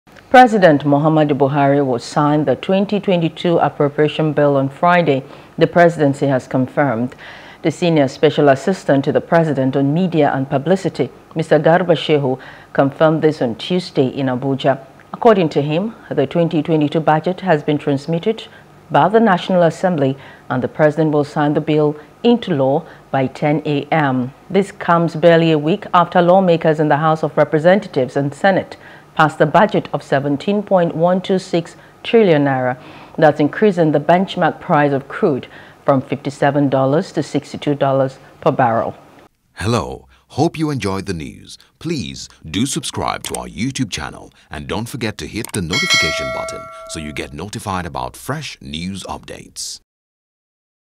2022: Buhari To Sign Budget Friday | NEWS
President Muhammadu Buhari will sign the 2022 Appropriation Bill on Friday, the Presidency has confirmed.